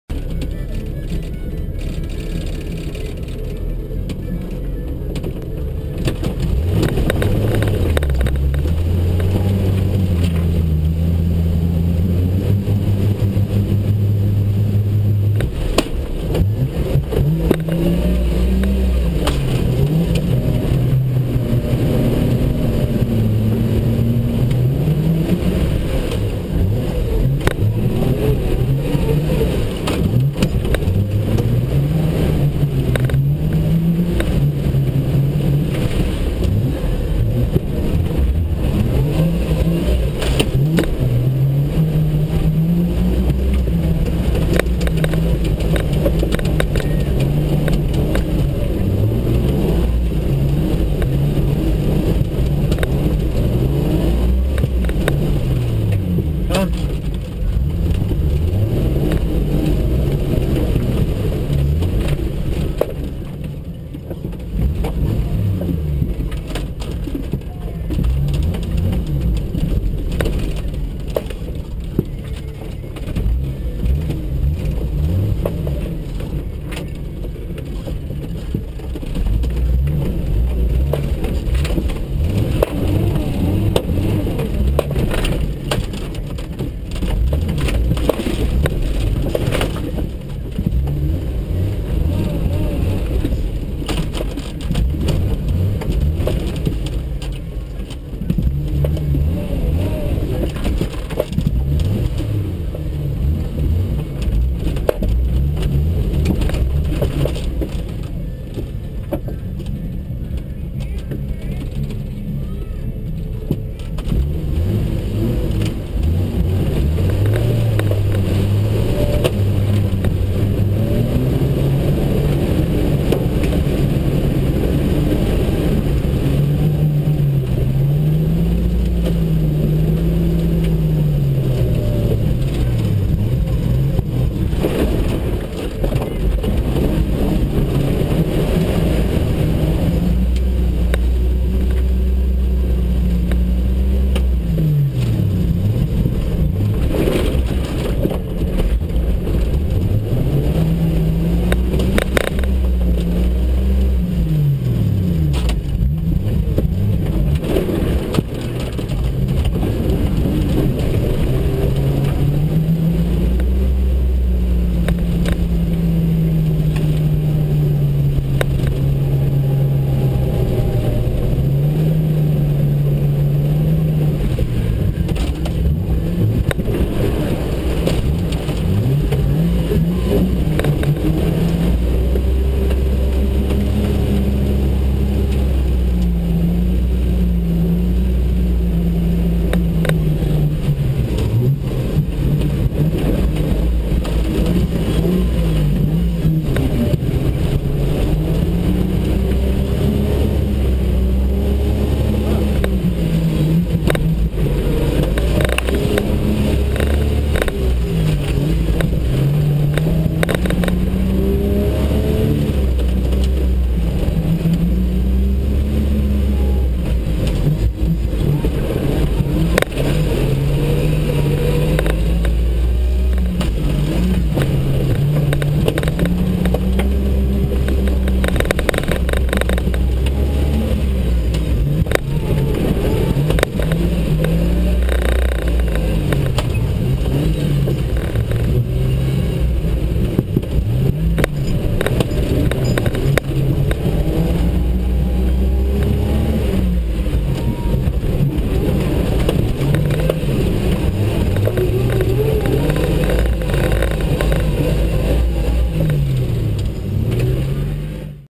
IVY Mud
Composite video from my phone & GoPro from one of the XJ's on the Ivy Ride on April 6 2013.  This is an area that Hatfield and McCoy off road trails are turning into a full size vehicle off road playground in West Virginia.  Ride put together by some folks from the WV Jeep Club (jeepinwv.com) but not an official members only ride